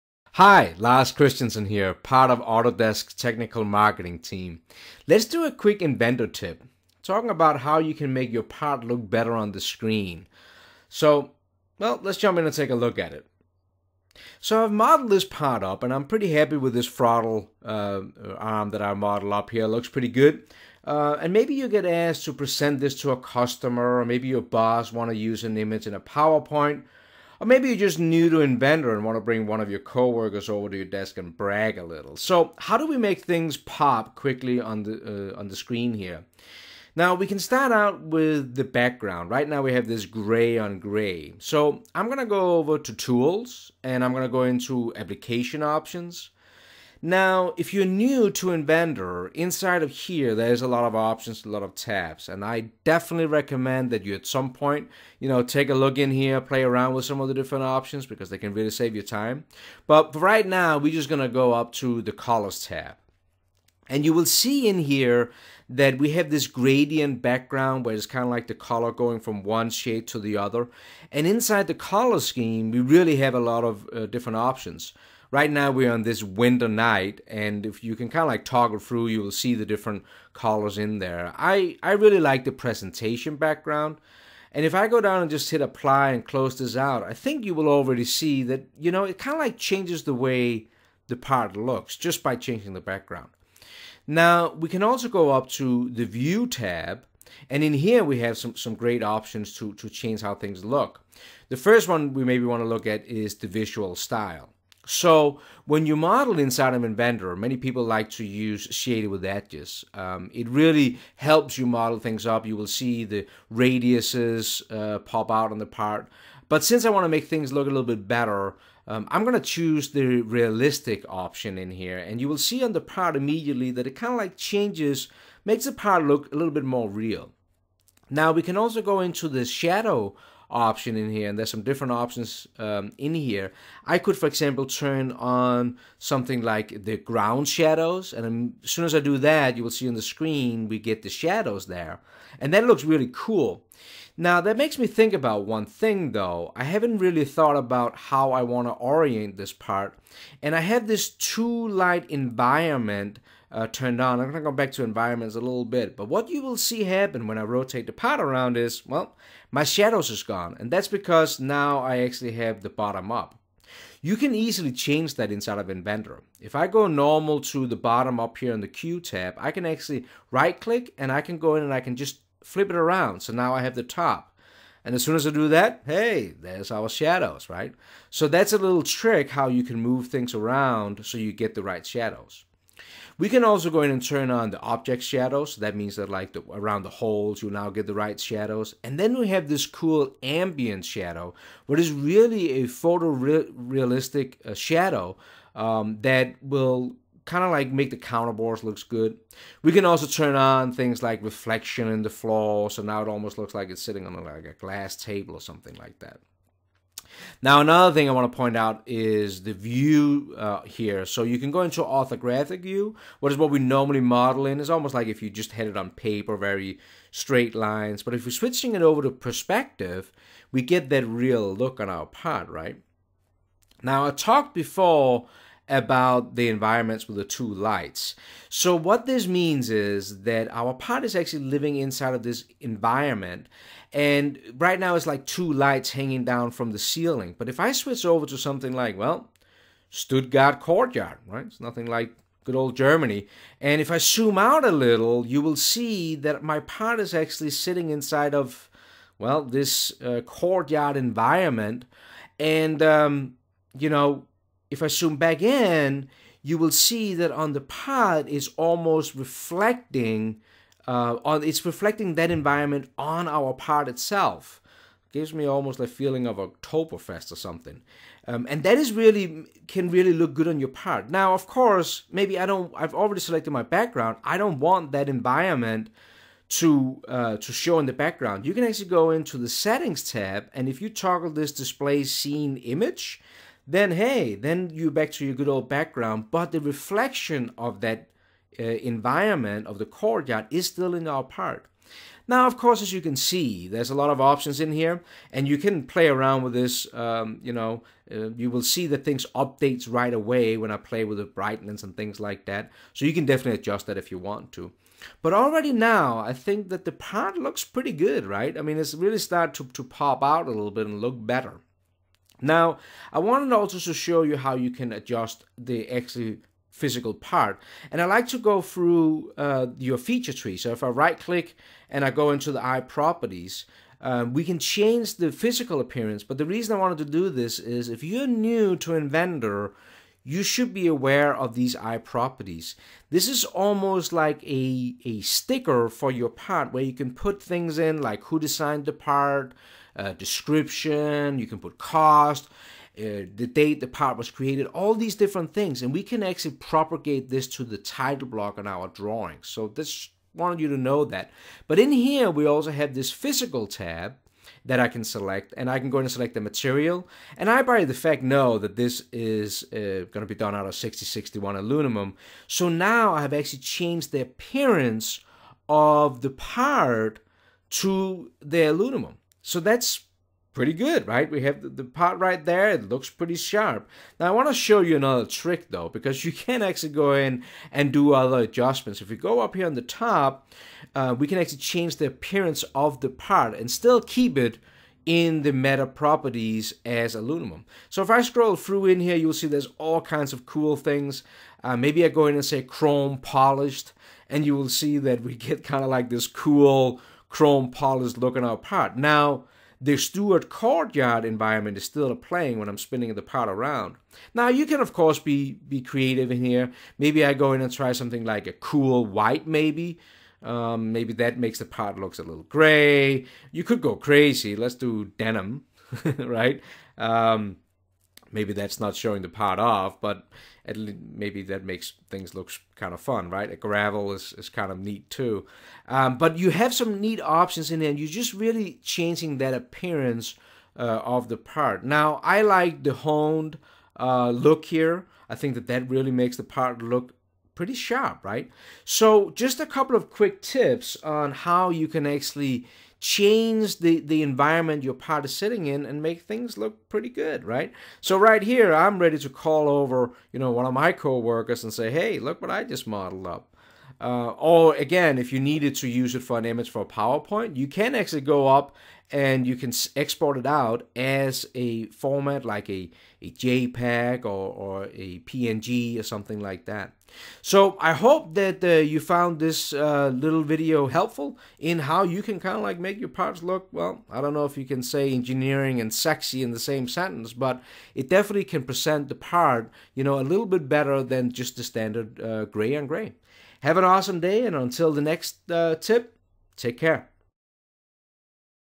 Two Minute Tip - Bringing Sexy Back to Inventor Parts
Autodesk Inventor Guru Lars Christensen brings the sexy back into your Inventor parts. You don't have to live with gray on gray, apply shadows, materials, reflections, make your parts as real on screen as they are on your products. No... It's not 2 minutes either.